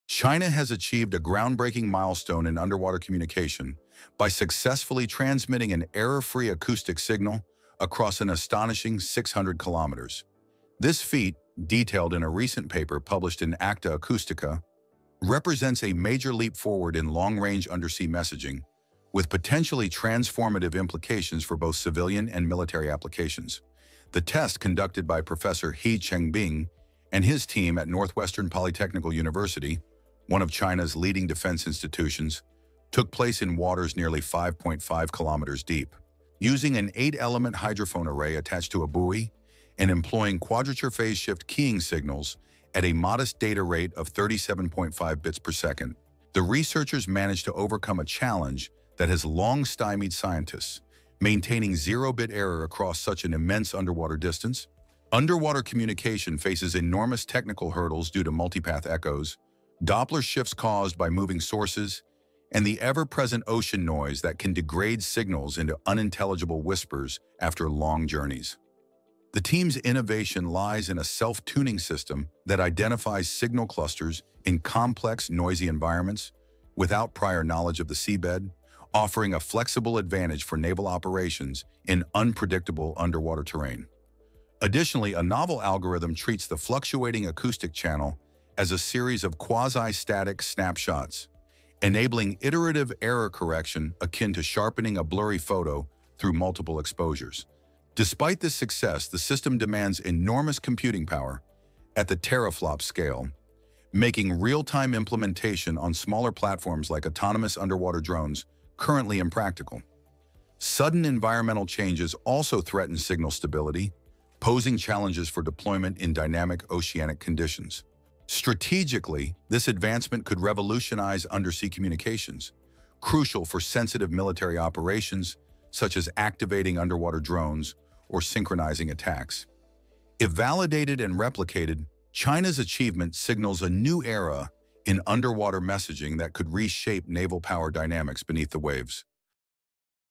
China Sends a Perfect Submarine Signal Across 600 km—and Straight Toward U.S. Interests
In a test conducted at sea, Chinese researchers say they have achieved what has long eluded scientists in underwater communication: error-free acoustic transmission over a distance of 600 kilometers.